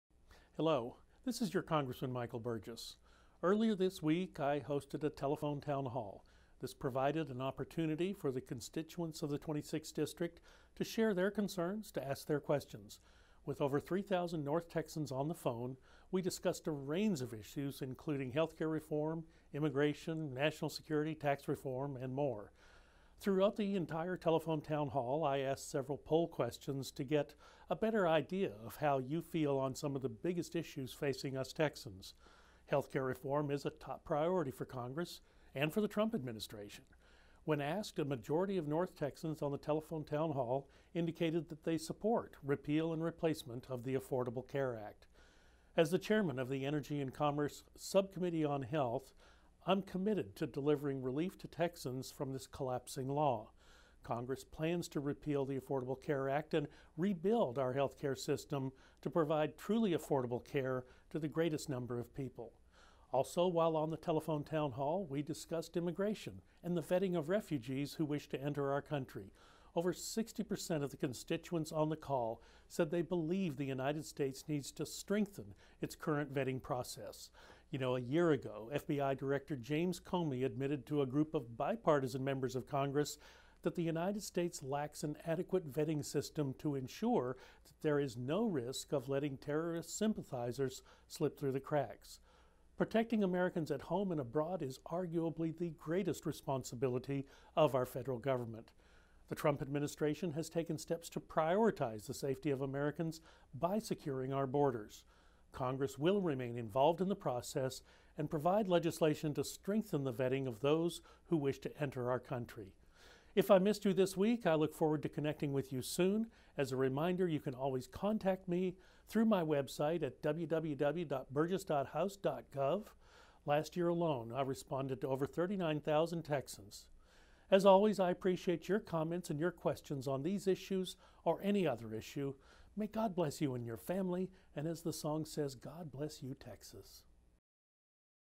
Weekly Video Address: Hearing from TX-26
Dr. Burgess discusses some topics of great importance to the constituents of the 26th district brought up on his most recent telephone town hall. With over 3,000 North Texans on the phone, there was a productive discussion on topics including health care reform, immigration and national security.